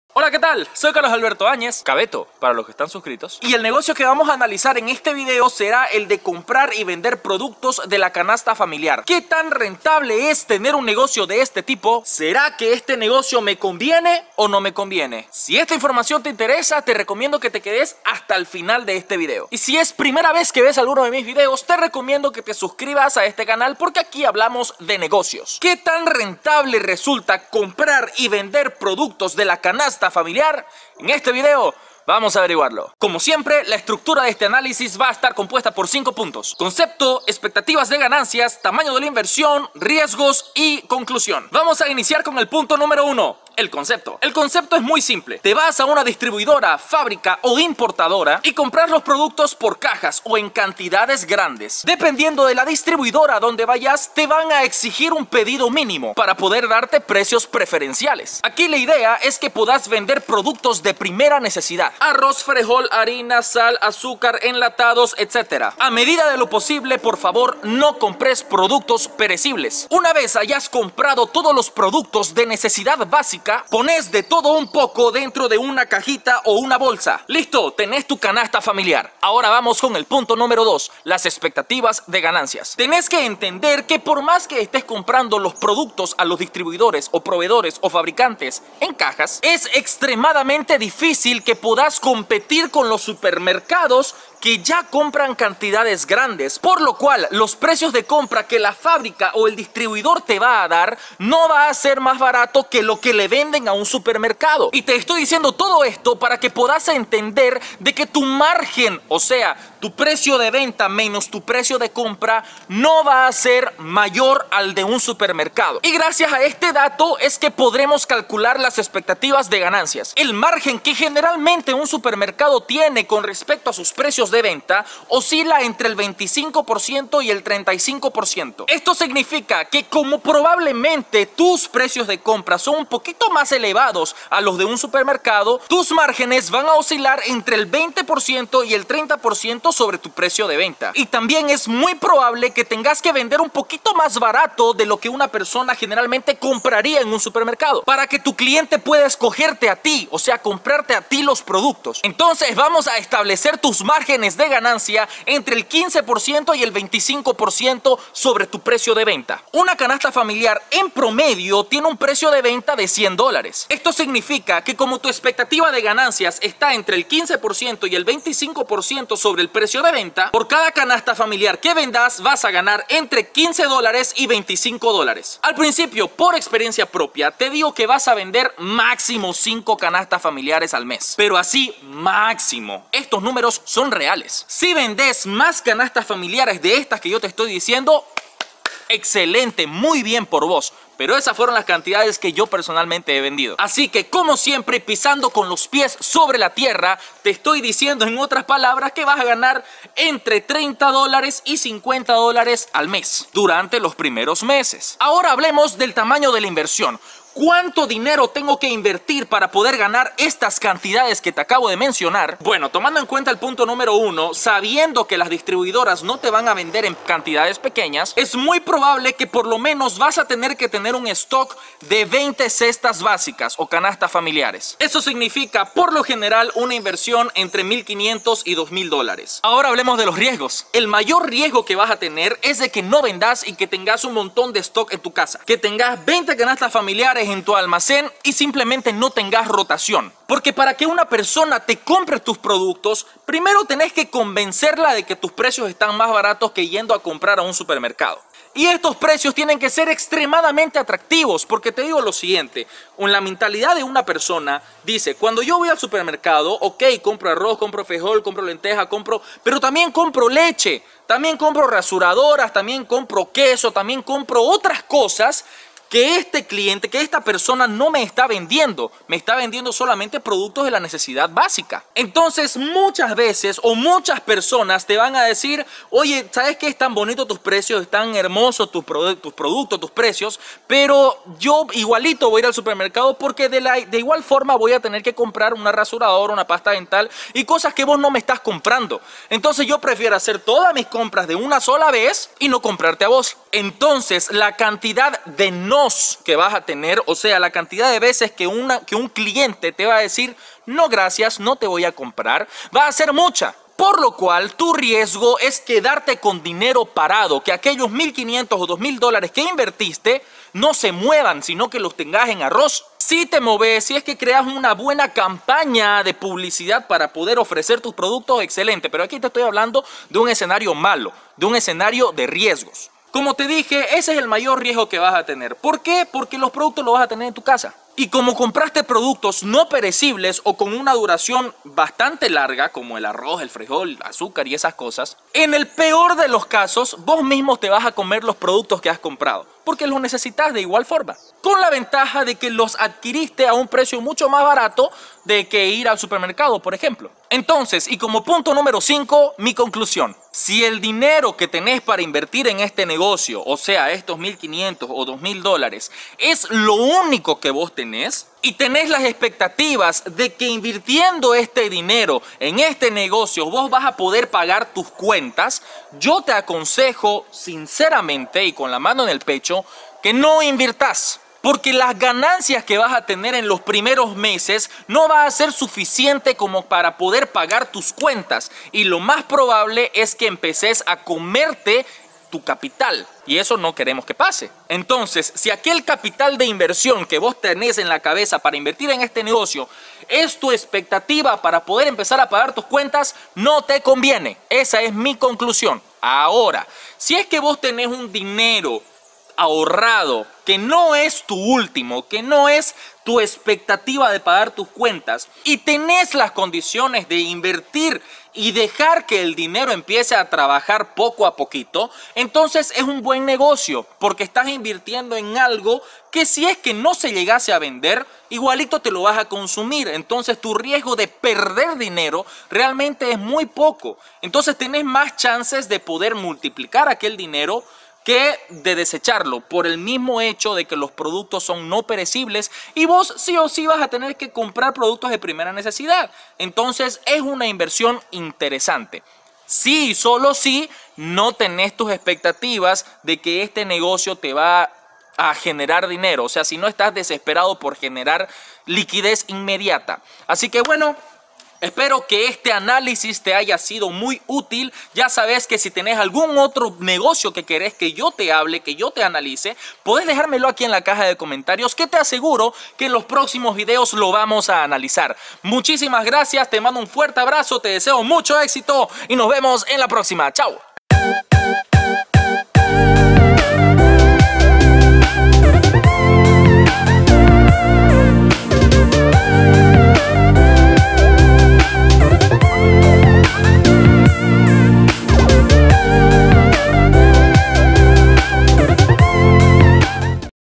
Analizando Negocios #2 | Comprar y Vender productos de la CANASTA FAMILIAR
Vamos a analizar el negocio de comprar y vender productos de la canasta familiar... En este video te mostraré cuáles son las ventajas y desventajas de iniciar un emprendimiento de este tipo (primera necesidad), basado en mi propia experiencia.
Puede llegar a ser un negocio rentable, desde que le dediques tiempo y esfuerzo en hacerlo crecer.
Iniciar un negocio no es simple, es un reto que todos los emprendedores tenemos... pero estoy seguro de que todo esfuerzo tiene su recompensa.
¡Te deseo mucho éxito en todo lo que hagas y espero que este video te sea útil!!
¡Un grande abrazo!